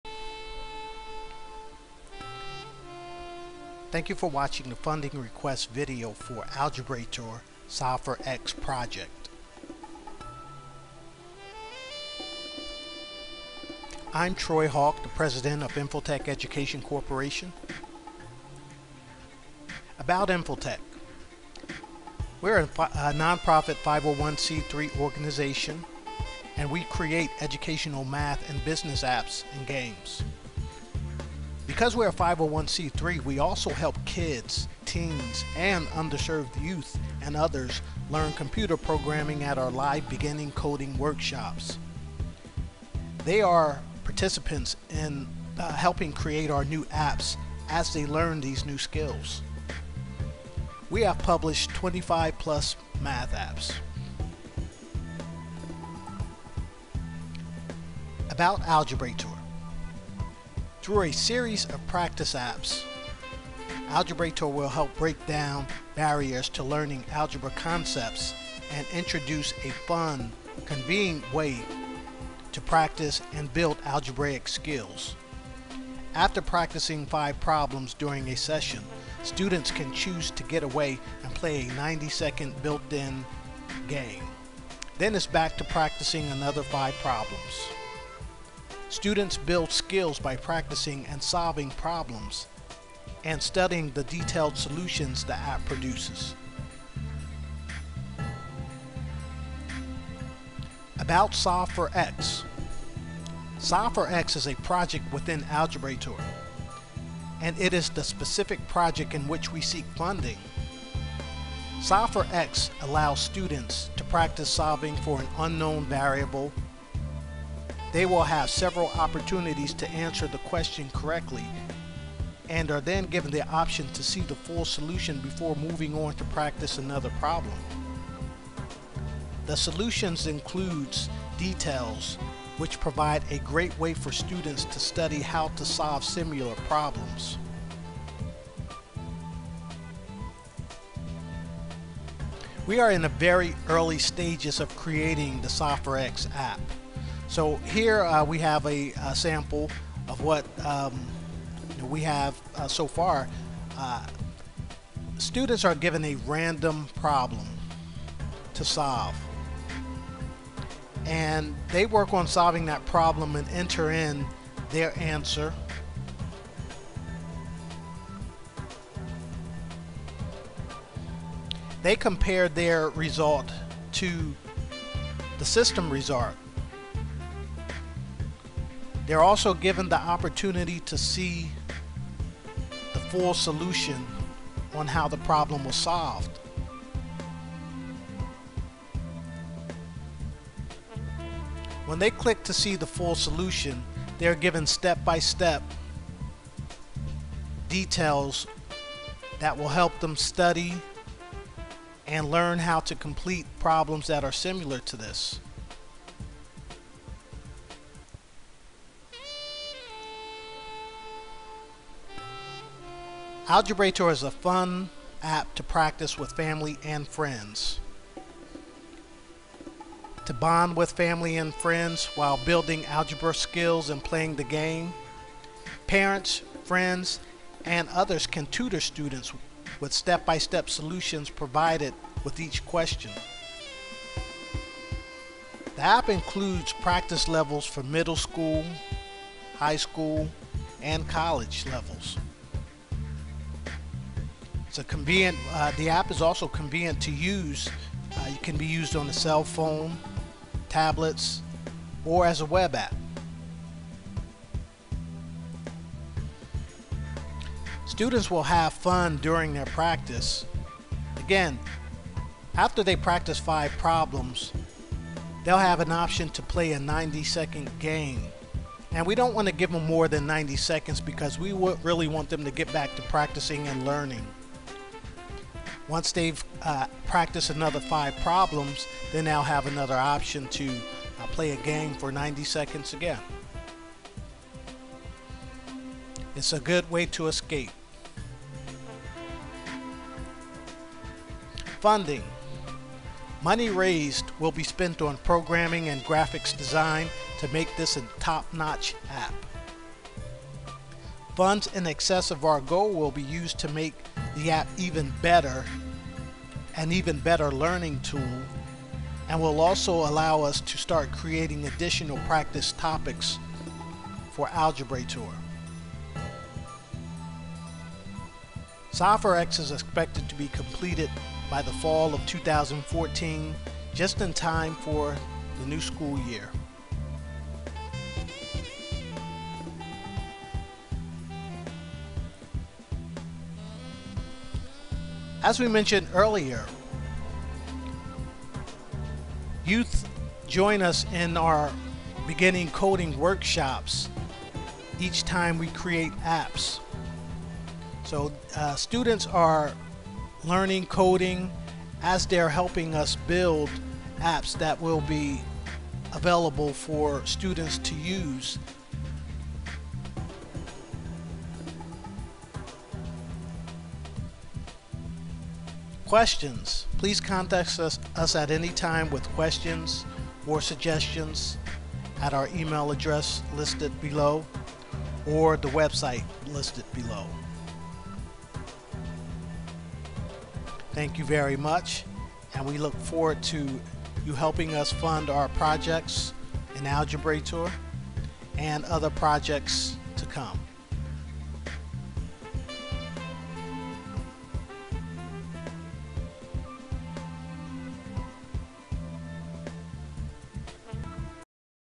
Algebraitor: "Solve for X" algebra app project
This video is a introduction to the Algebraitor: Solve for X project,  funding request co build the app.

Solve for X is an app that will allow students to practice solving for a variable, but will also include a gaming element that will allow students to take a small break from practicing.

This app will allow students to improve their algebraic skills, by practicing and studying the detailed solutions provided for each question.

App will be available for smart phones, tablets, and as a web app.

InfoTech Education Corp. is a 501c3, nonprofit org.